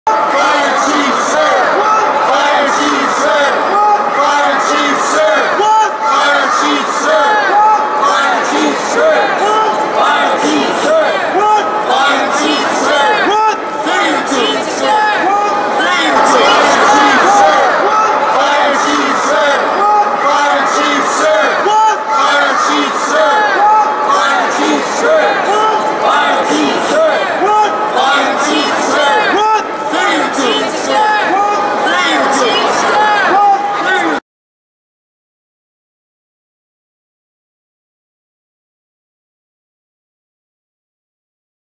Fillmore Bay Area Media Group Present Fire Chief Suhr
Protesters At City Hall To Talk With San Francisco Mayor Ed Lee About Firing Suhr